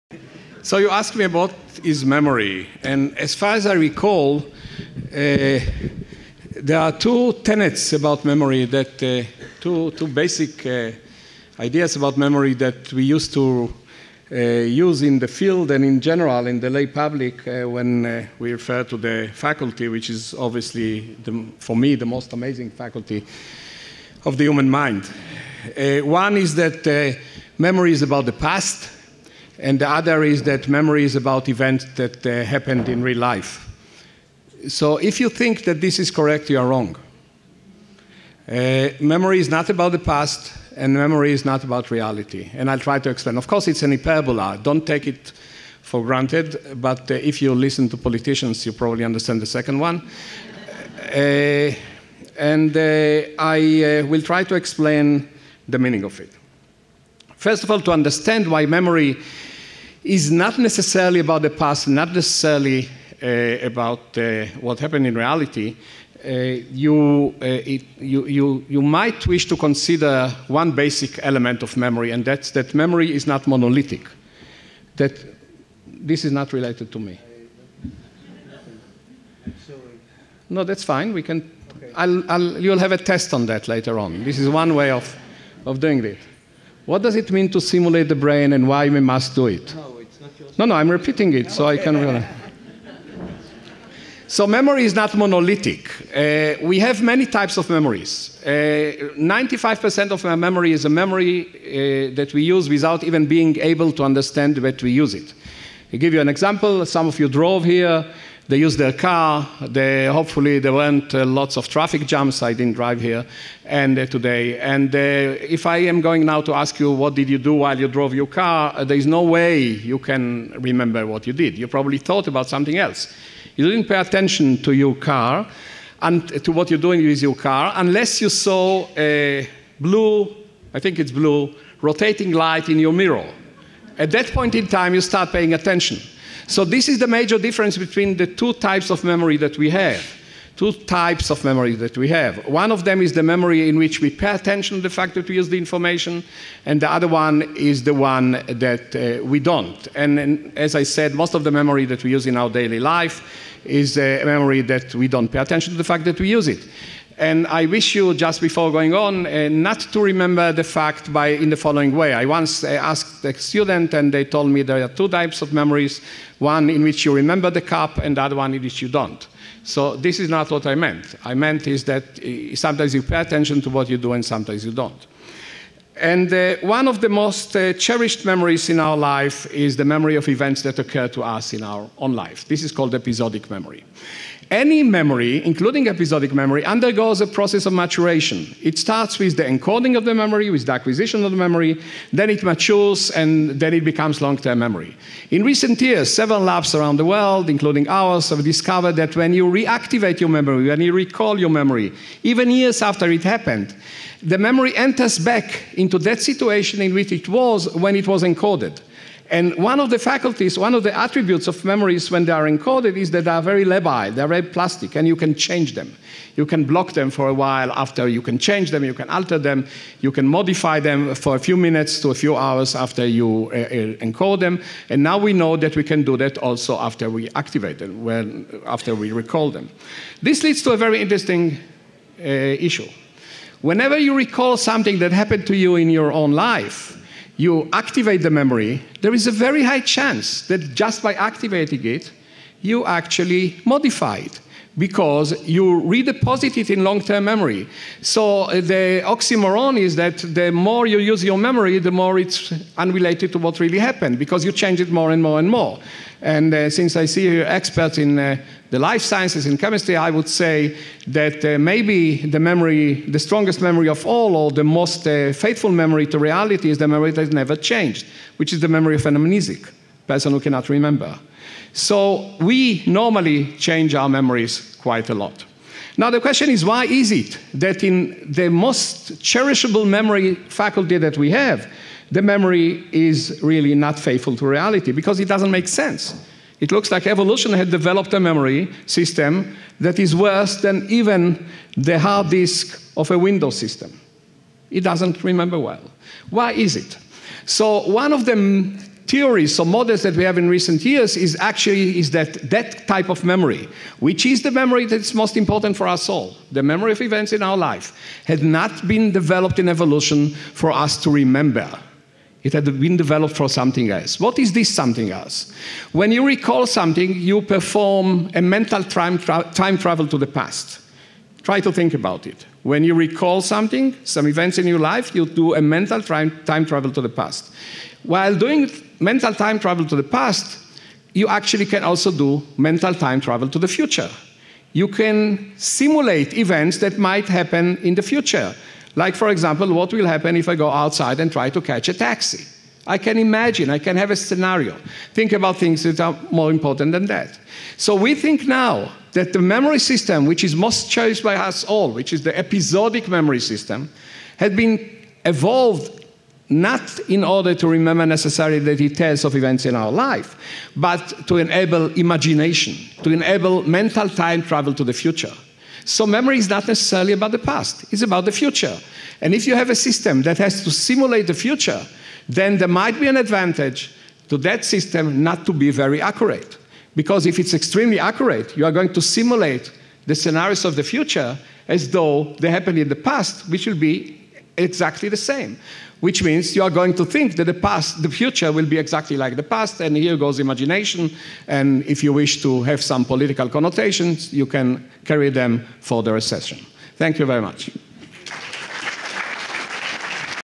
Brain Research -- The Wondrous Voyage into Ourselves - Professor Yadin Dudai
The fifth Israeli Presidential Conference opening ceremony:

Science is grappling with the great enigma of the human brain and how it operates. We expect the human brain to solve the puzzle of itself. Europe is focused on the "Human Brain Project", the Obama Administration intends to invest billions in brain research, and Israeli scientists too are throwing themselves into the task.  How much progress have we made in understanding the brain and what still remains to be discovered? Are dramatic discoveries likely to change the basic understandings about human nature that have prevailed since the dawn of history?  Will it be possible to improve how well the brain functions and to significantly affect human behavior? What new ethical questions does brain research raise?